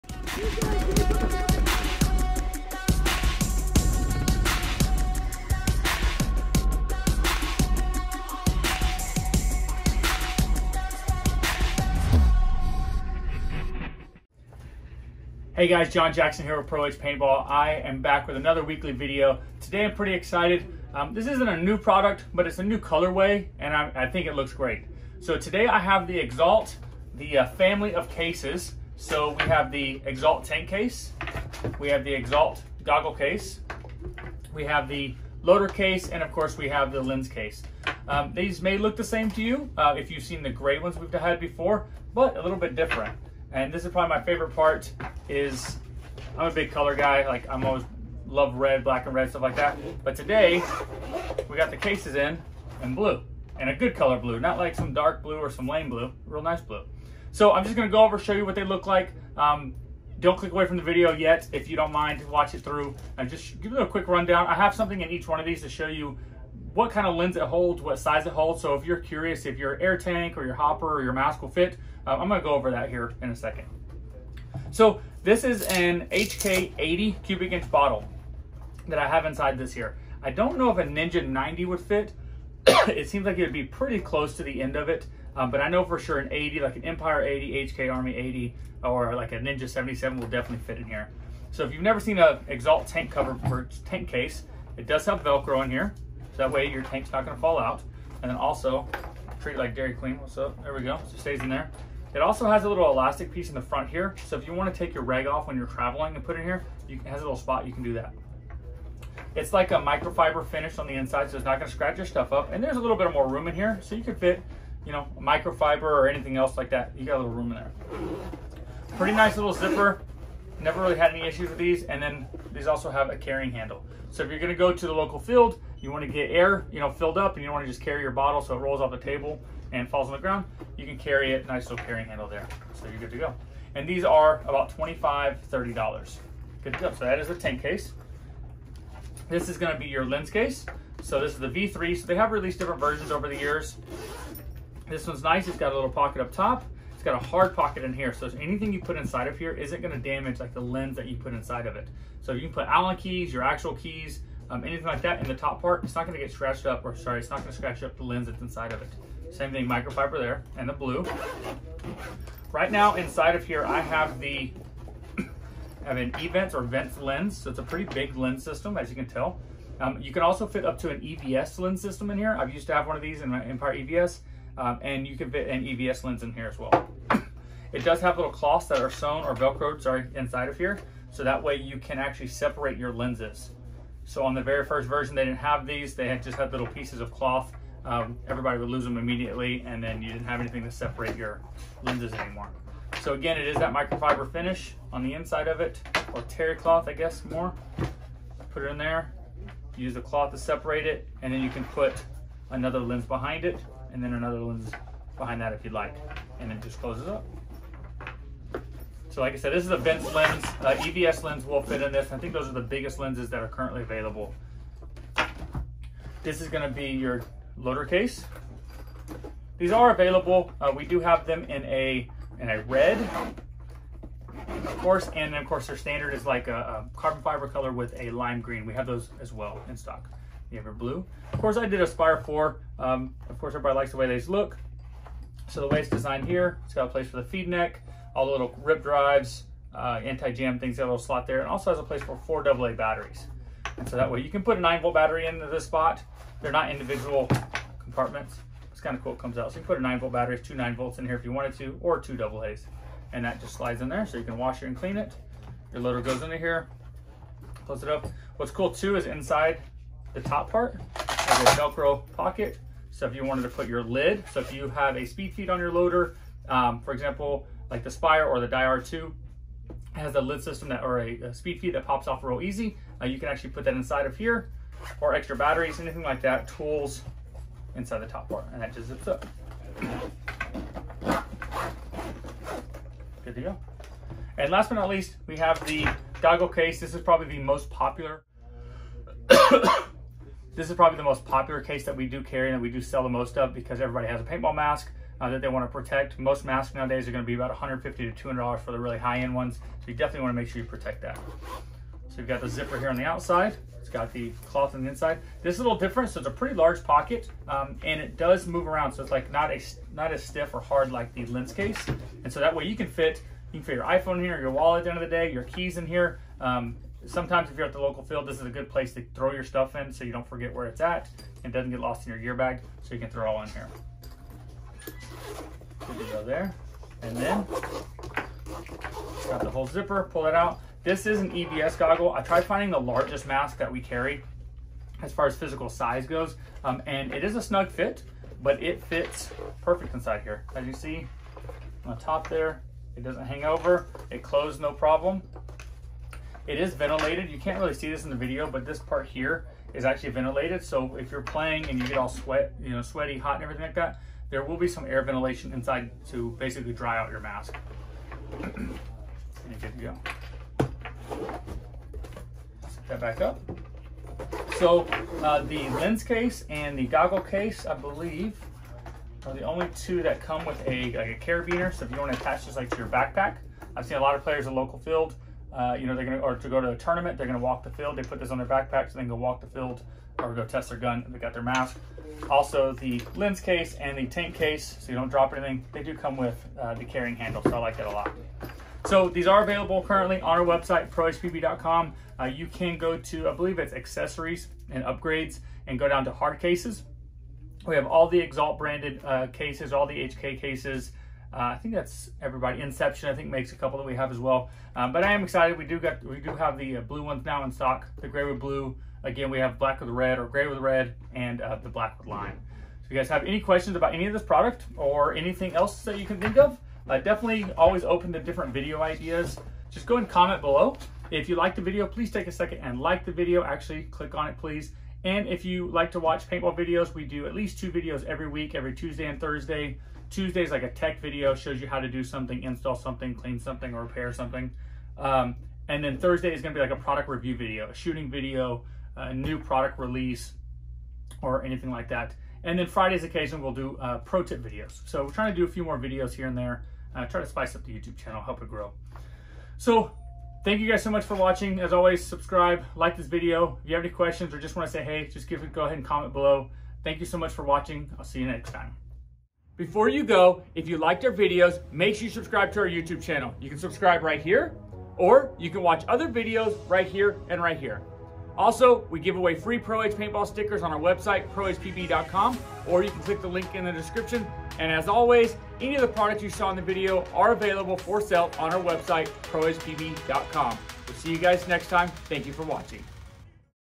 New Blue Exalt Paintball Cases - Now Available at ProEdgePB.com
Exalt paintball cases are some of the most popular cases and most widely used cases currently.

They are now available in a new Blue inside color way and Grey carbon fiber outside design.

Looking for more product protection?

Shop all of our paintball gear bags and back packs